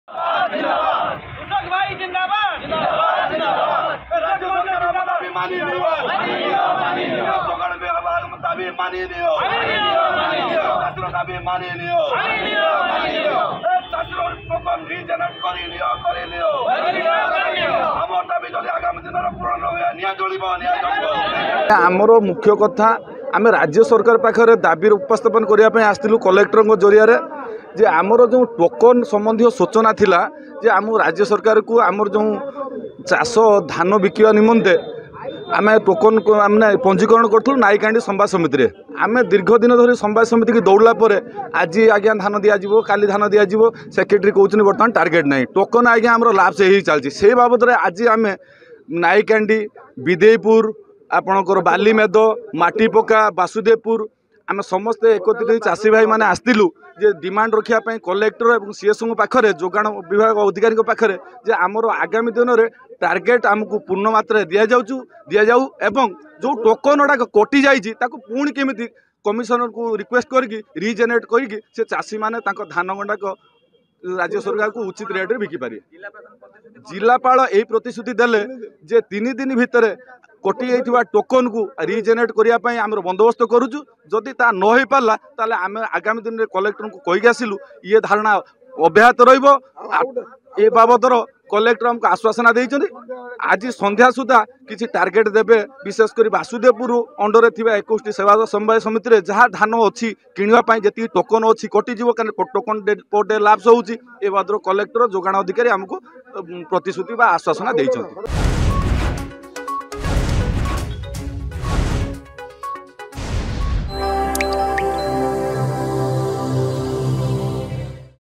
ଭଦ୍ରକ ଜିଲ୍ଲାପାଳଙ୍କ କାର୍ଯ୍ୟାଳୟ ସମ୍ମୁଖରେ ଧାରଣାରେ ଚାଷୀ
ଭଦ୍ରକ ଜିଲ୍ଲାରେ ଶତାଧିକ ଚାଷୀଙ୍କ ଶହ ଶହ କୁଇଣ୍ଟାଲ ଧାନ ରହିଛି  । ଟୋକେନ ଲାପ୍ସ ହେବା ସହ ମଣ୍ଡିରେ ଧାନ କ୍ରୟ ହୋଇ ନ ପାରିବାରୁ ଏହି ପ୍ରତିବାଦ କରି ଆଜି ବାସୁଦେବପୁର ସମନ୍ବିତ କୃଷକ ମଞ୍ଚ ତରଫରୁ ଭଦ୍ରକ ଜିଲ୍ଲାପାଳଙ୍କ କାର୍ଯ୍ୟାଳୟ ସମ୍ମୁଖରେ ଚାଷୀ ମାନେ ଧାରଣା ରେ ବସିଛନ୍ତି ।